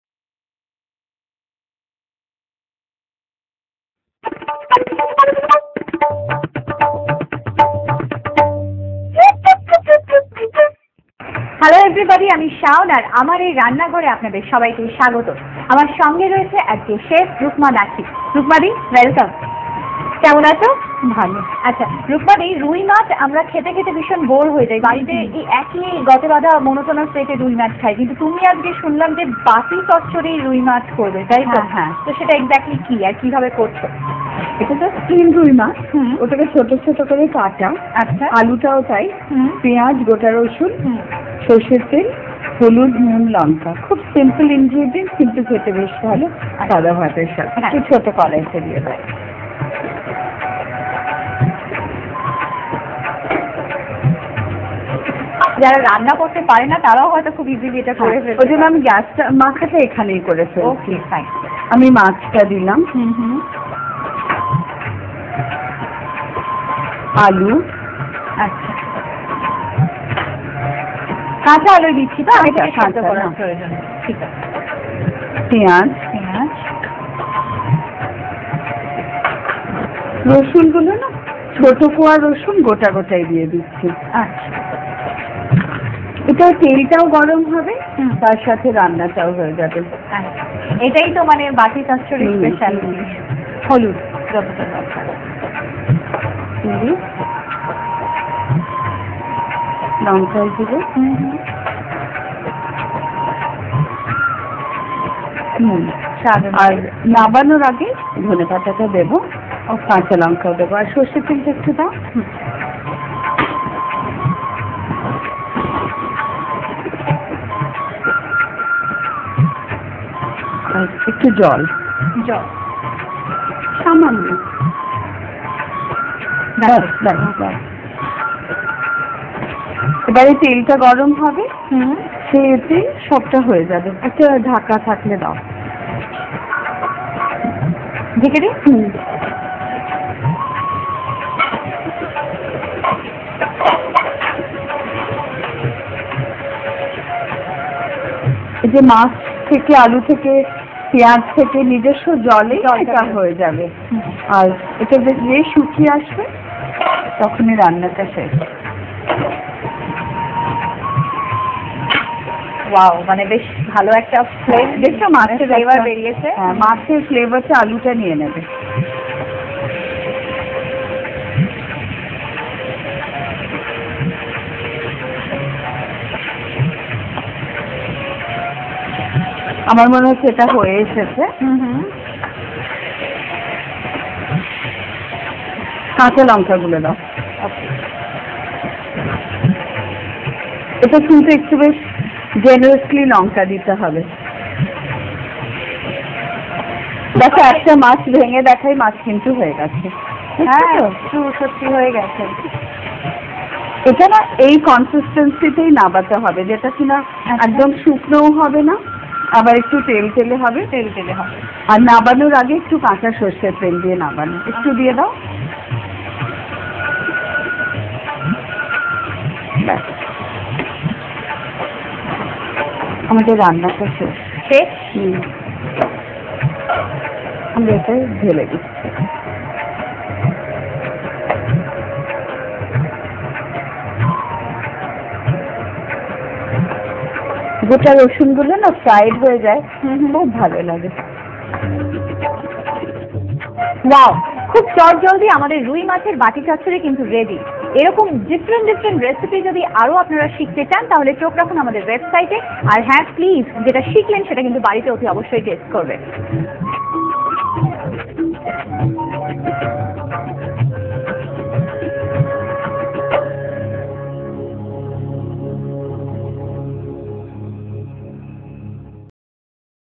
Bengali Rui Machher Bati Chochori Recipe
This is how one can cook Rui Maach Bati Chochori at home. Find more recipes for delicious Bengali dishes